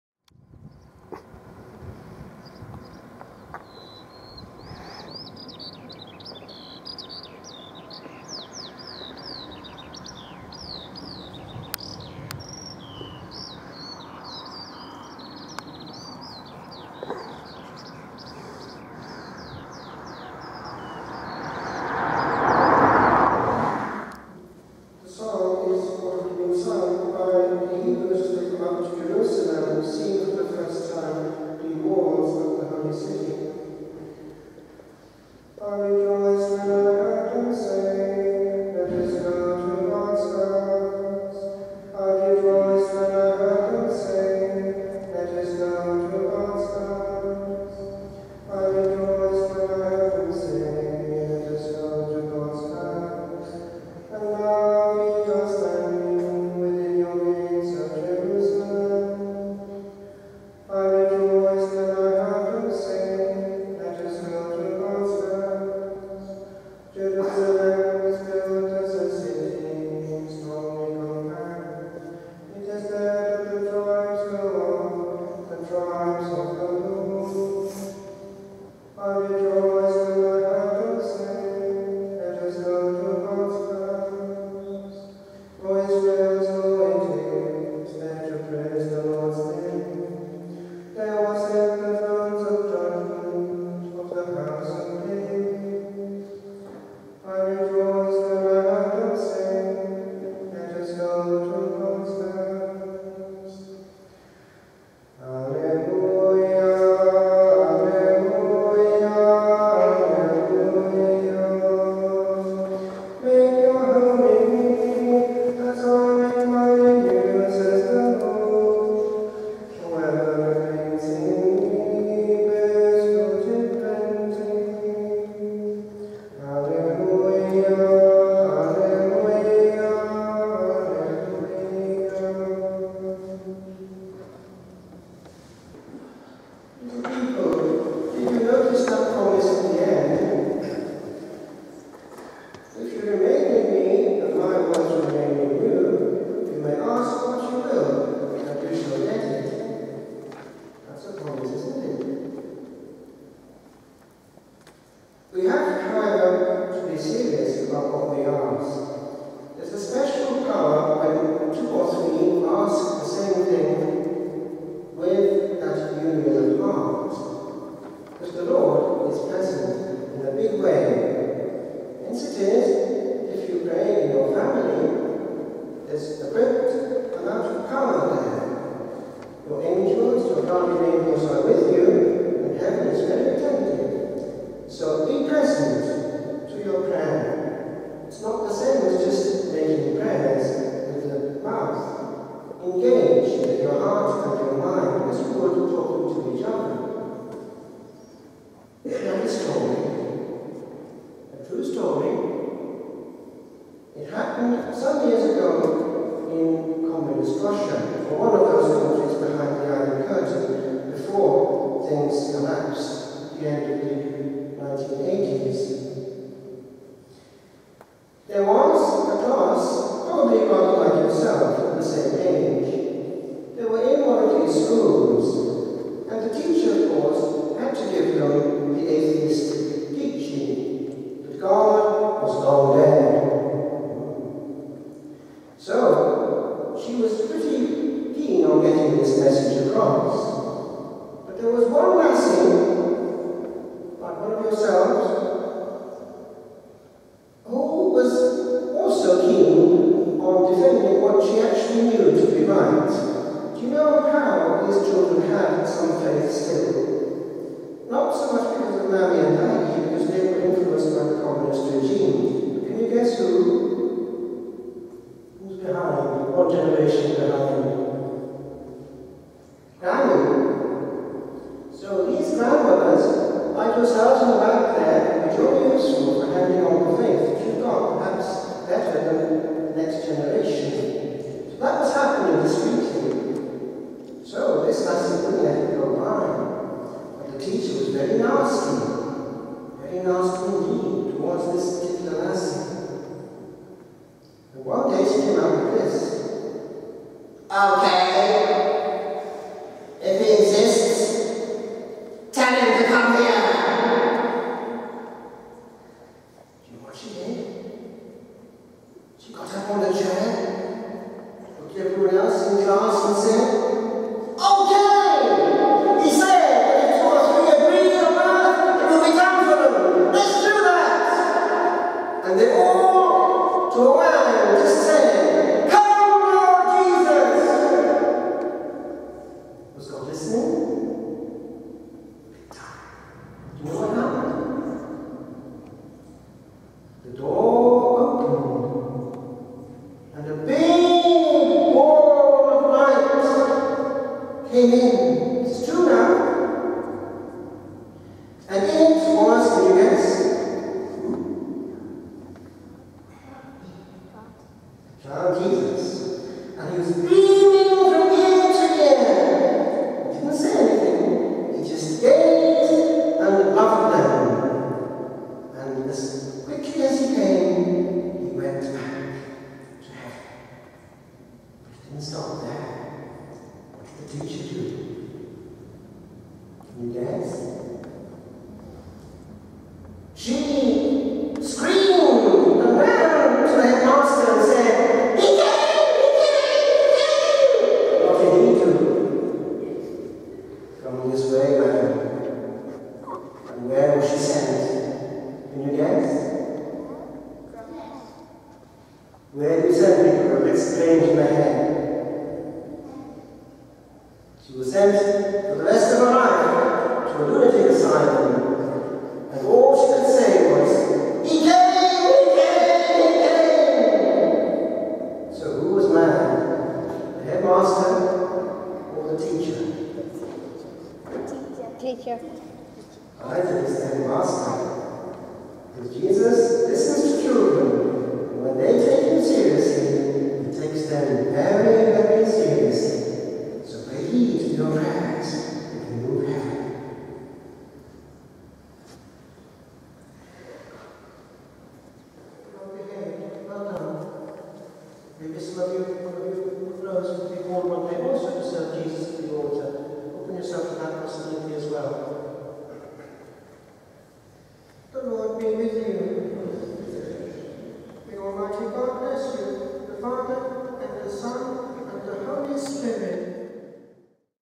Fr. David Jones - 21st May 2014, Children's Mass
Mass in Duleek  with children being prepared for first Holy Communion